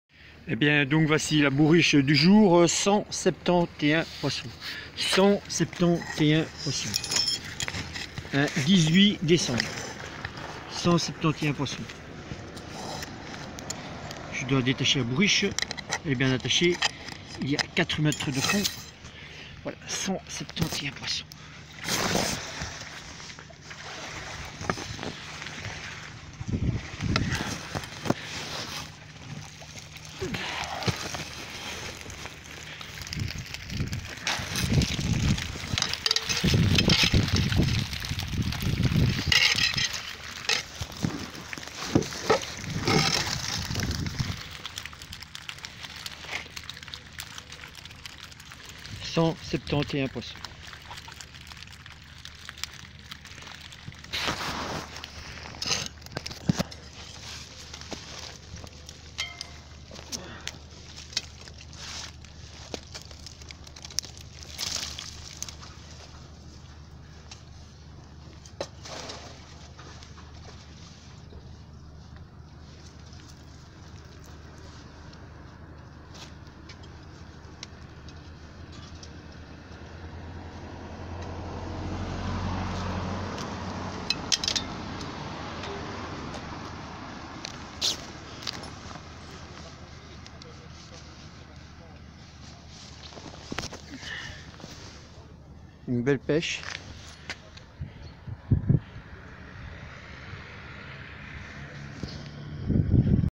bourriche 18 décembre 2020 +++ 171 poissons en 6 h 00
tout a été capture avec des perles en quelques heures
Pour obtenir des perles clic ci dessous